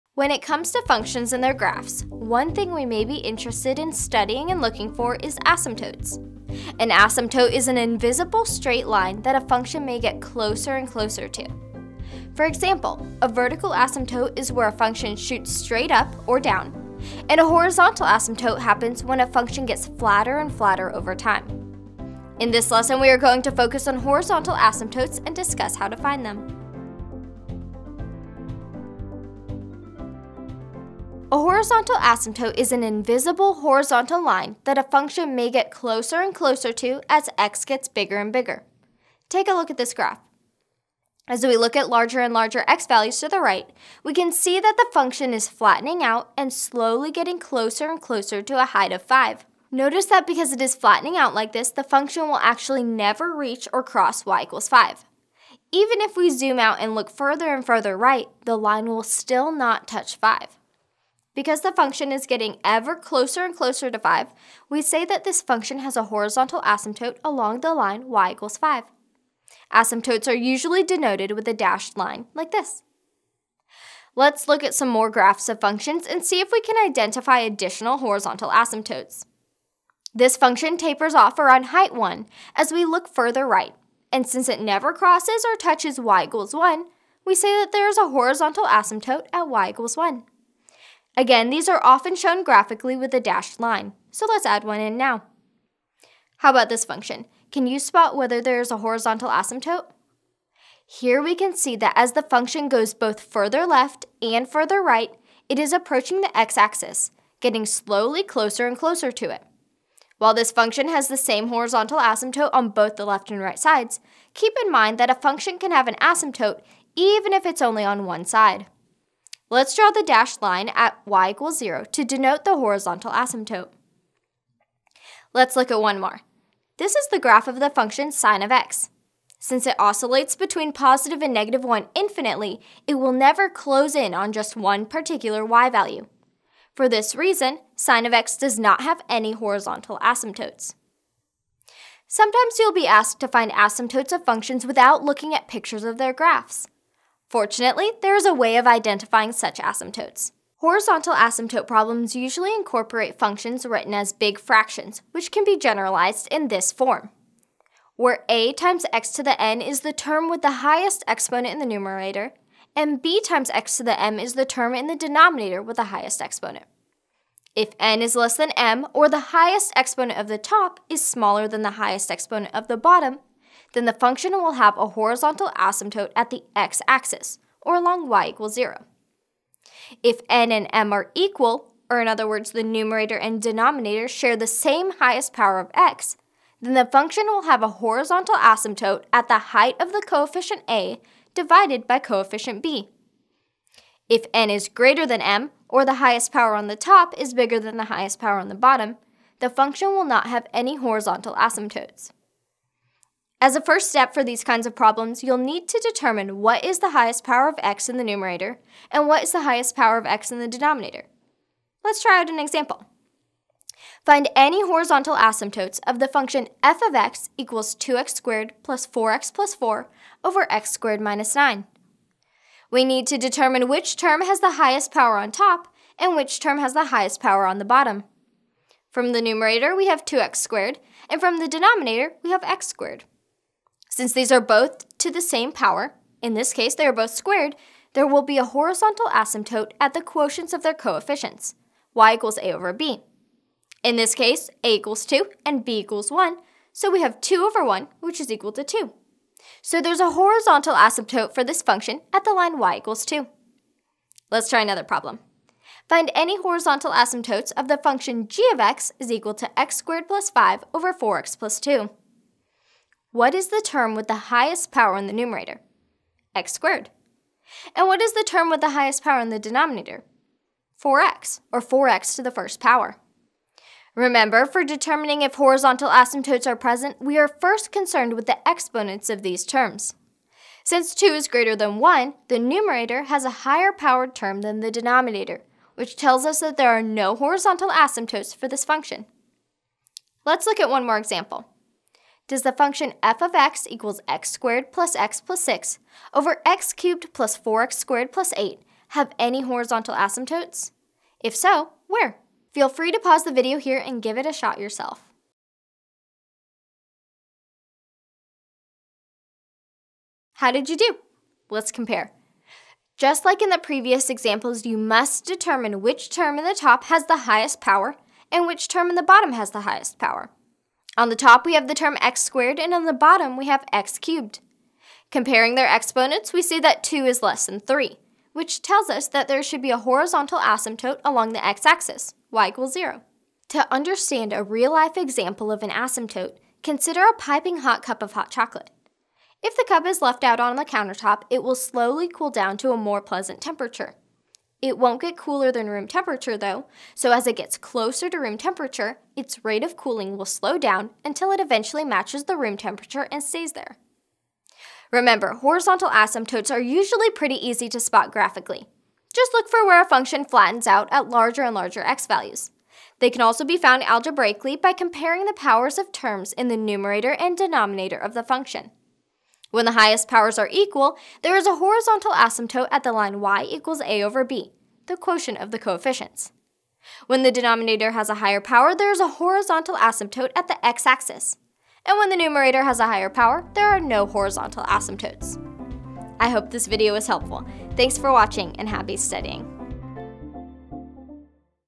Horizontal Asymptotic Notation
When it comes to functions and their graphs, one thing we may be interested in studying and looking for is asymptotes. An asymptote is an invisible straight line that a function may get closer and closer to. For example, a vertical asymptote is where a function shoots straight up or down, and a horizontal asymptote happens when a function gets flatter and flatter over time. In this lesson, we are going to focus on horizontal asymptotes and discuss how to find them.